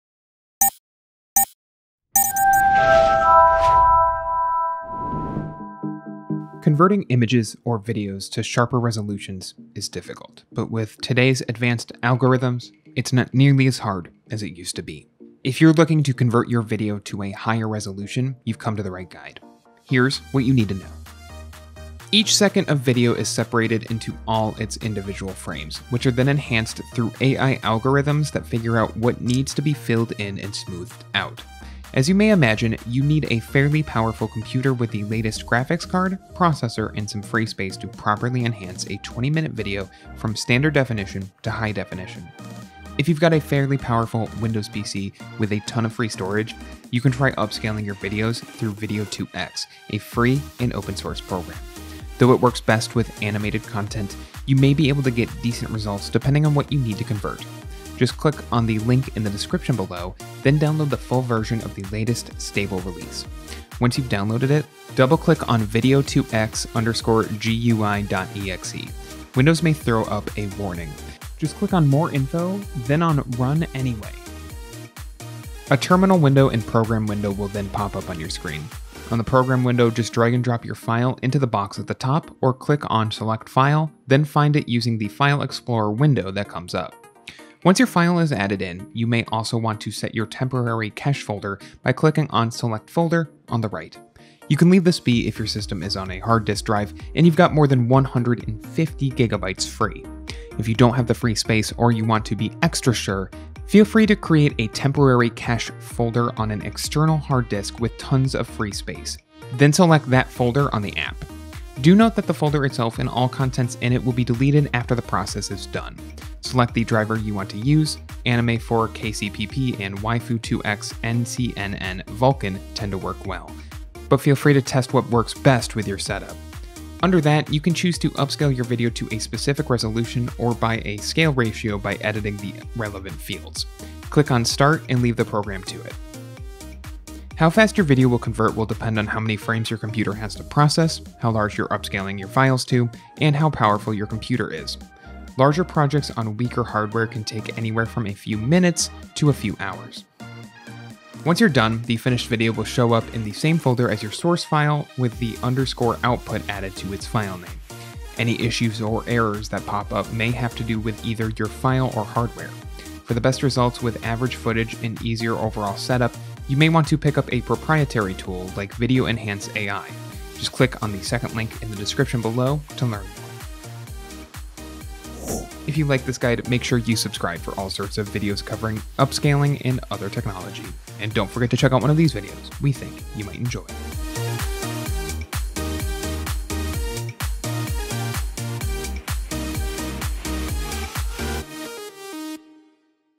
How to Convert a Video to HD
Converting images or videos to sharper resolutions is difficult, but with advanced algorithms, it’s not nearly as hard as it used to be. If you’re looking to convert your video to a higher resolution, you’ve come to the right guide. Here’s what you need to know!